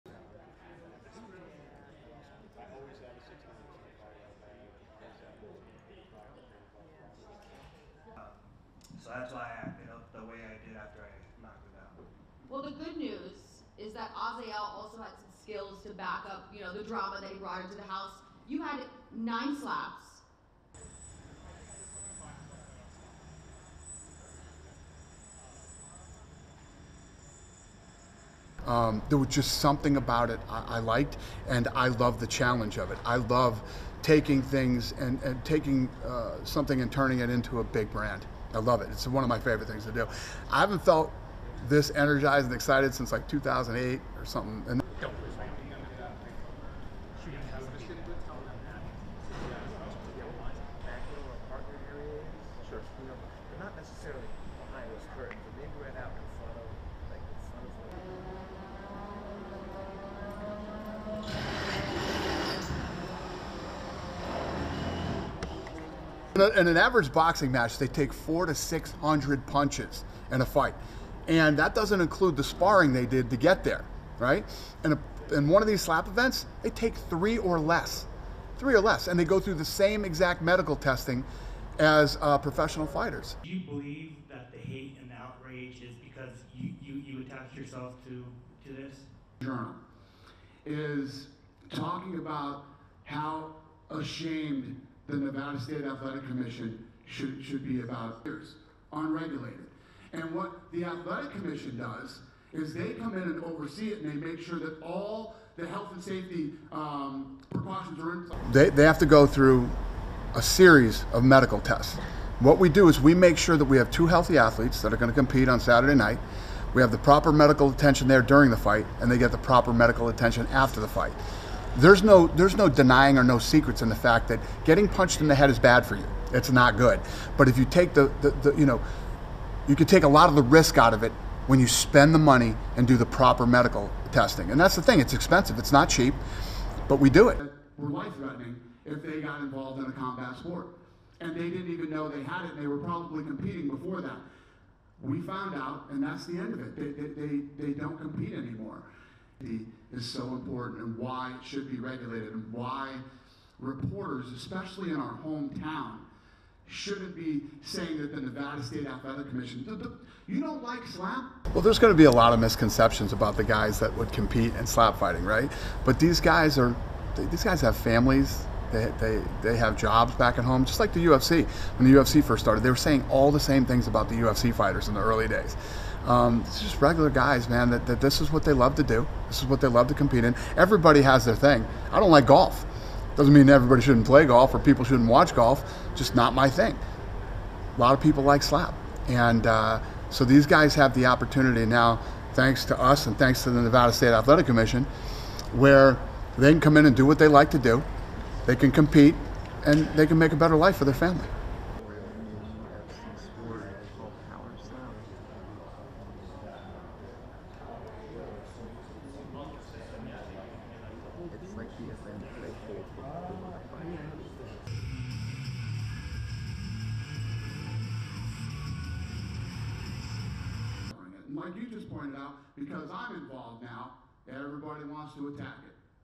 UFC: Slap flighting is next big combat sport
RESTRICTION SUMMARY: CLEARED FOR USE BY SNTV AND AP CLIENTS
ASSOCIATED PRESS
Las Vegas, Nevada – 8 March 2023
1.  Banner of upcoming match
2. Power Slap logo
3. UFC logo
4. SOUNDBITE (English) Dana White, UFC President:
“There was just something about it I liked and I loved the challenge of it. I love taking things and taking something and turning it into a big brand. I love it. It's one of my favorite things to do. I haven't felt this energized, excited since like 2008 or something.”
5. Power Slap arena sign.
6. Power Slap stands
7. Power Slap stage
8. SOUNDBITE (English) Dana White, UFC President:
“In an average boxing match, they take 4 to 600 punches in a fight. And that doesn't include the sparring they did to get there. Right. And then one of these slap events, it takes three or less, three or less. And they go through the same exact medical testing as professional fighters.”
9.  Ron Bata on stage (right)
10. Darius Mata-Varona on stage (center)
11. Ayjay Hintz on stage (right)
12: SOUNDBITE (English) Dana White, UFC President:
“They have to go through a series of  medical tests. What we do is we make sure that we have two healthy athletes that are going to compete on Saturday night. We have the proper medical attention there during the fight and they get the proper medical attention after the fight. There's no there's no denying or no secrets in the fact that getting punched in the head is bad for you. It's not good. But if you take the you know, you can take a lot of the risk out of it when you spend the money and do the proper medical testing. And that's the thing. It's expensive. It's not cheap, but we do it.”
13. Slap fight press conference, wide
14. Vernon Cathey on stage
15. SOUNDBITE (English) Dana White, UFC President:
“Well, there's going to be a lot of misconceptions about the guys that would compete and stop fighting. Right. But these guys are these guys have families that they they have jobs back at home, just like the UFC and the UFC first started. They were saying all the same things about the UFC fighters in the early days. It's just regular guys now that this is what they love to do. This is what they love to compete in. Everybody has their thing. I don't like golf. It doesn't mean everybody shouldn't play golf or people who watch golf. Just not my thing. A lot of people like Slap. And so these guys have the opportunity now. Thanks to us and thanks to the Nevada State Athletic Commission, where they come in and do what they like to do. They can compete and they can make a better life for their family.”
16. Banner for upcoming fight
17. Azael Rodriguez (left)
18. Wide shot Slap fight press conference.

STORYLINE:
The competitors stand rigidly upright with their hands behind their backs, waiting to absorb a brutal slap to the face.

When the open-handed blow is delivered, there's a sharp report and the reaction can be dramatic. Some fighters barely move, while others stumble backward or fall to the floor. Some are knocked out.

UFC President Dana White is selling slap fighting as the next big thing in combat sports, putting his money and the resources of one of the world's foremost mixed martial arts organizations behind the Power Slap League. The Nevada Athletic Commission has sanctioned the league for competitions in Las Vegas.

White and the competitors remain unfazed, comparing commentary on slapping to the negative reaction the UFC faced in its infancy more than 20 years ago.

But White is charging ahead.

And the fighters know if they lose the coin toss and get slapped first, it will hurt.